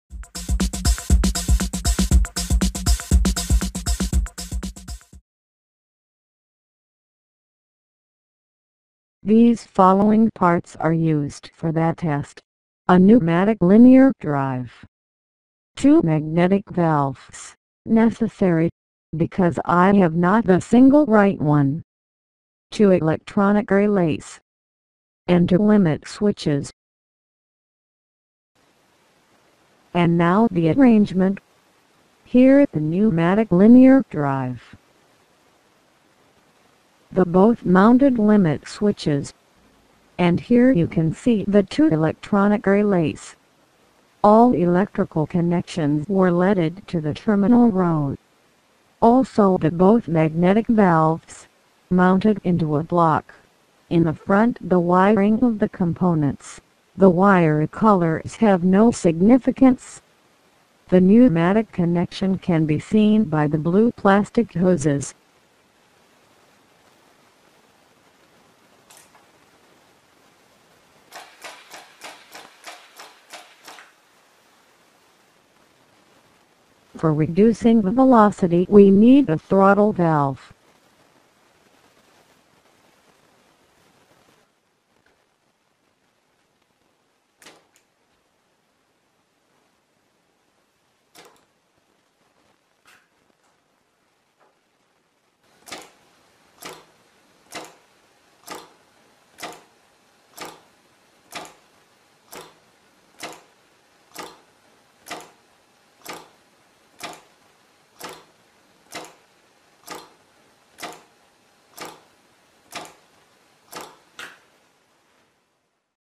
Festo linear drive
Trying to realize an alternating movement with a linear drive. The selection of ideal components was limited.